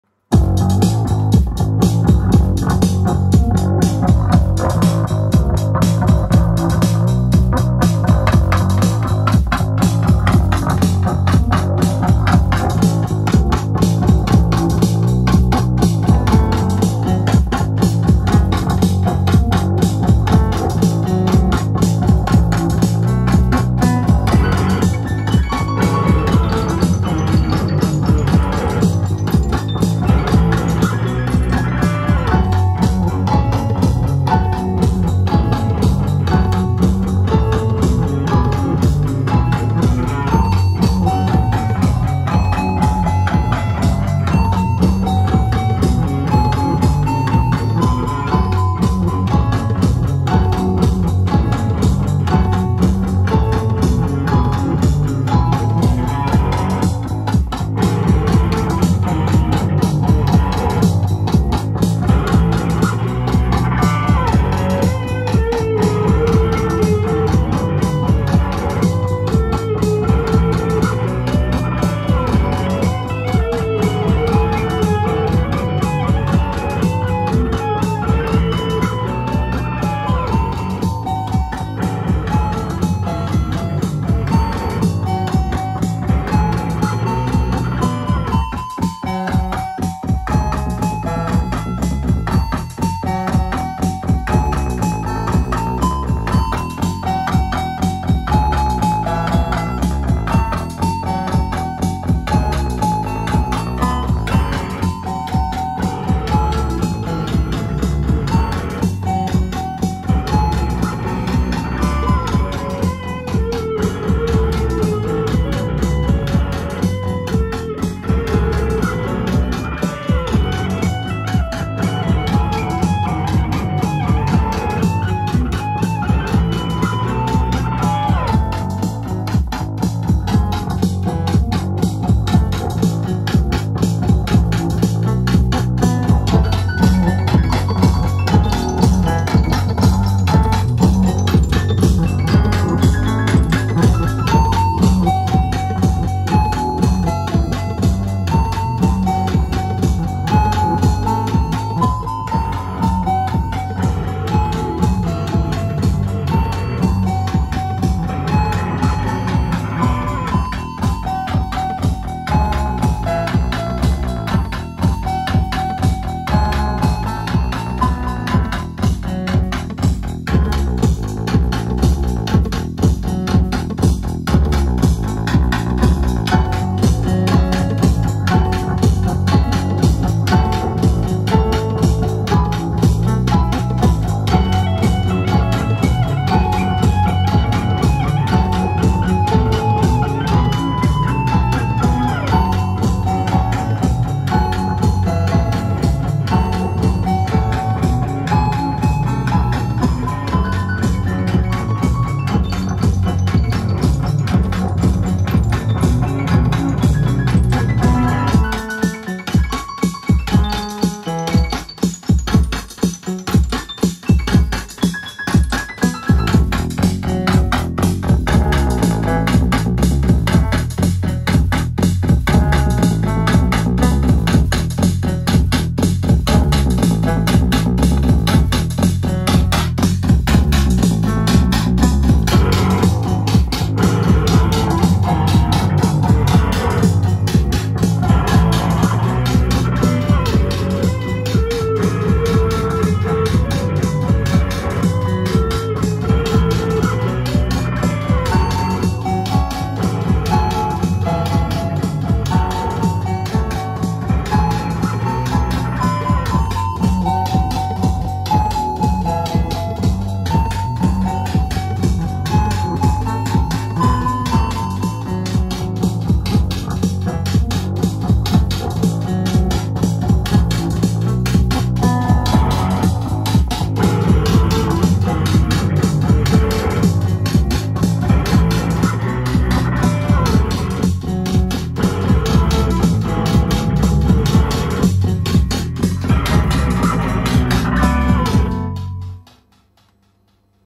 Hail Manowar
I happened to listen to Manowar whilst driving to the supermarket, hence the Behringer Crave line around which I added the rest. Just for fun, as usual.